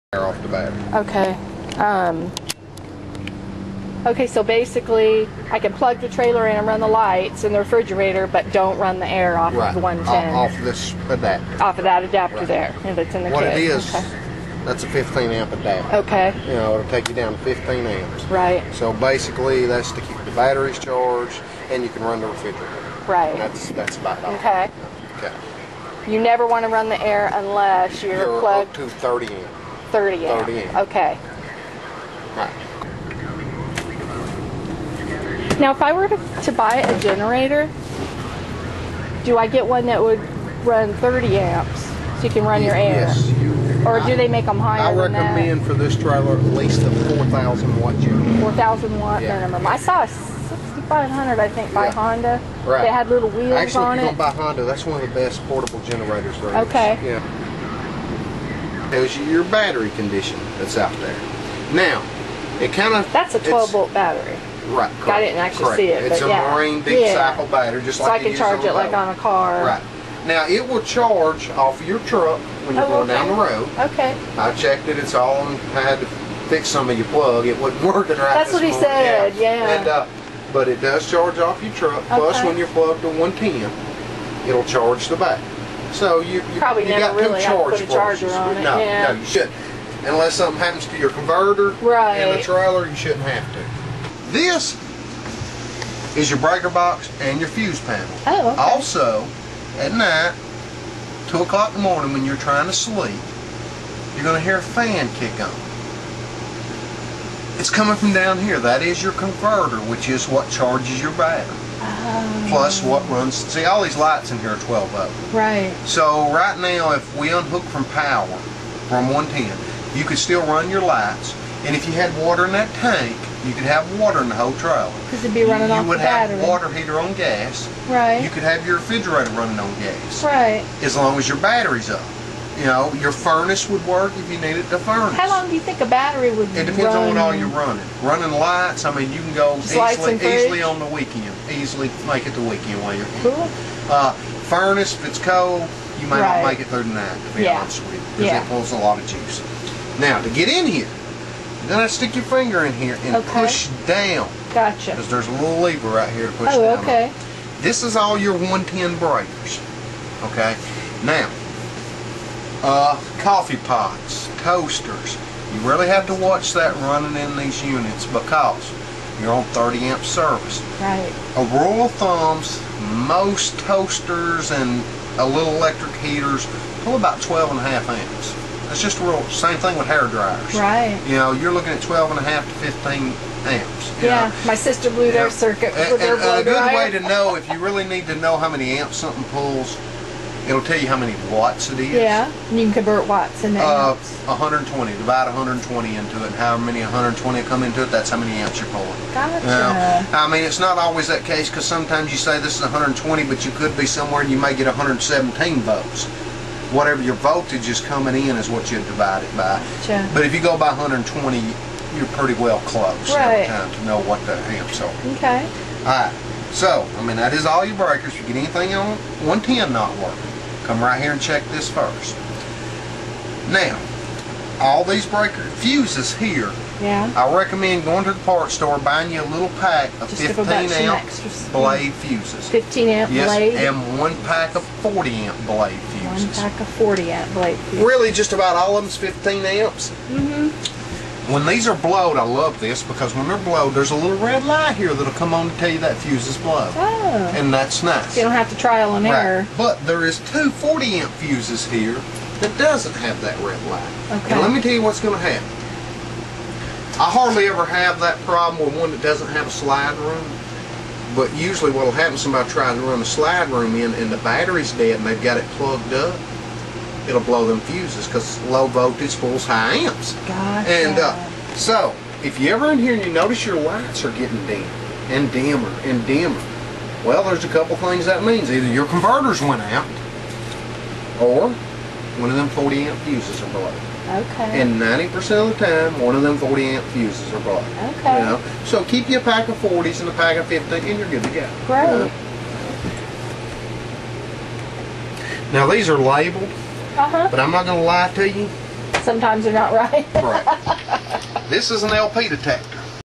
Travel Trailer Electrical
Okay, so this is dumb, but I video taped the guy explaining what to do with my new travel trailer.